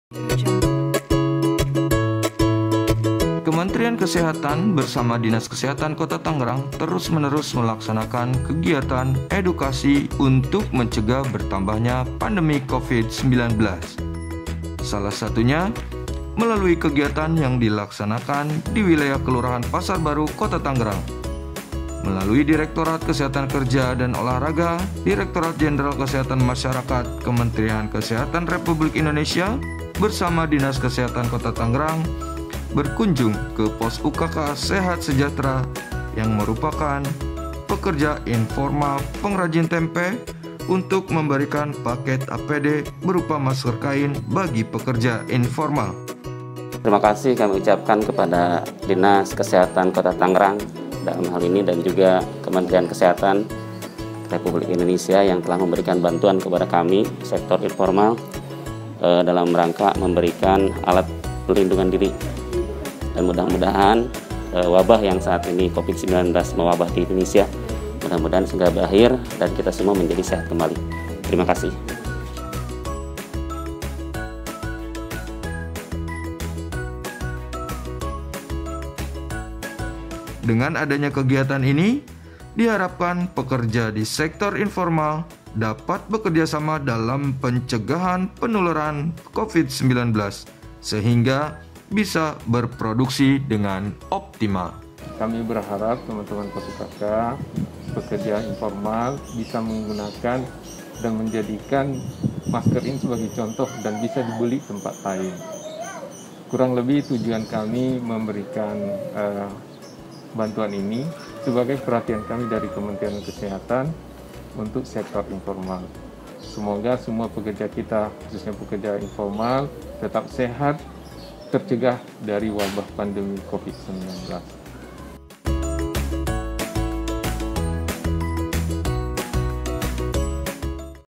Penyerahan Masker Kain untuk Pekerja Informal di Kota Tangerang
Dinas Kesehatan Kota Tangerang bekerjasama dengan Kementrian Kesehatan RI membagikan ribuan masker kain untuk mencegah penularan Covid 19 bagi pekerja sektor informal